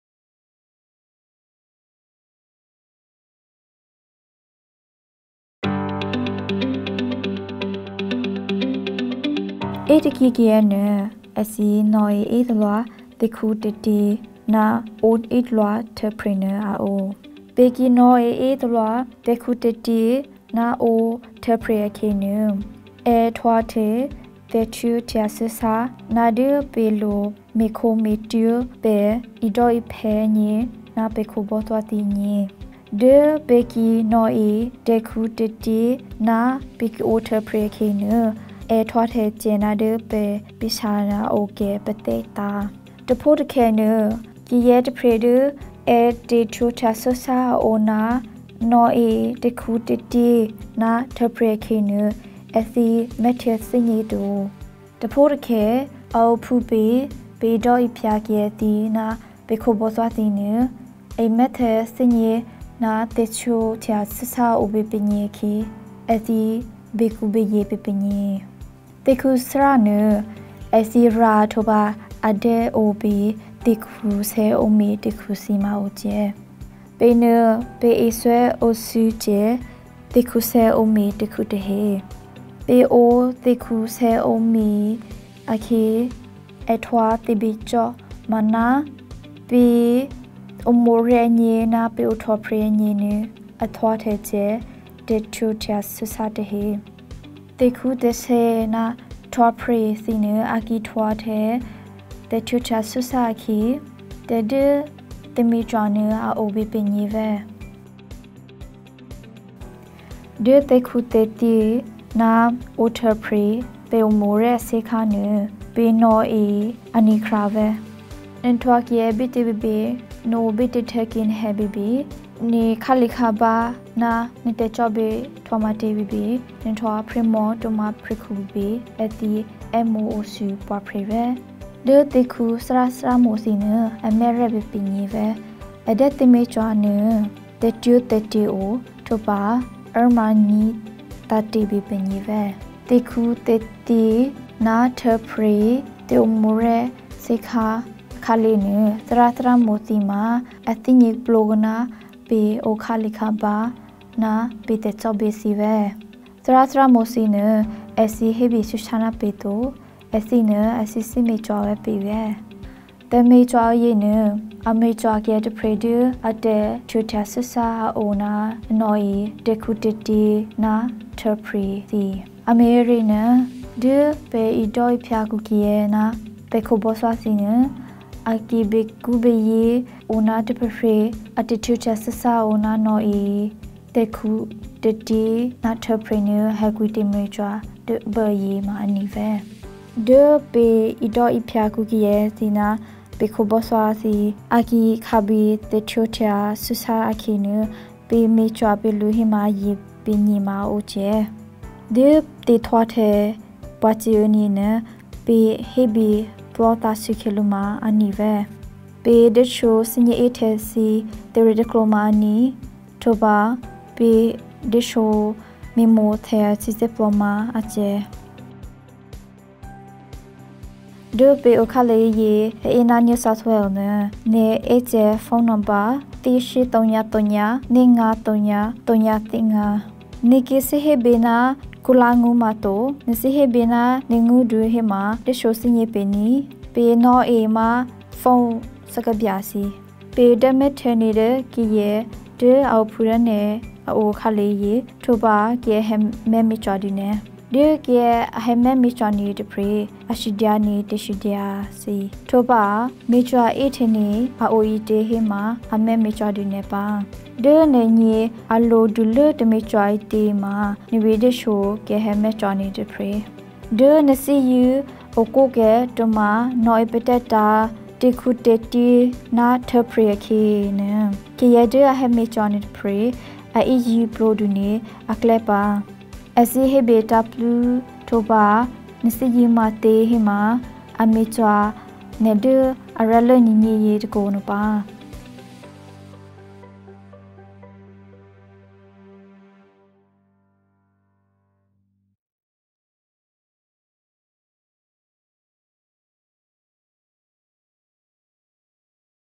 Karenni - Where can I get help when alcohol or drugs are a problem
Do you or someone you care about need help to stop or cut down alcohol or drug use? Alcohol and Drug Information Service ( ADIS) and Drug and alcohol services from Area Health Services are free, confidential and welcome people from all cultures. Professional interpreters are available if you need help to communicate with staff. Drug and Alcohol staff can also ask for an interpreter. The resource links to ADIS State-wide support and Illawarra Shoalhaven Local Health District Drug & Alcohol services. The resource shows people of various background getting support from these services either face to face, using an interpreter or linking with phone interpreting service.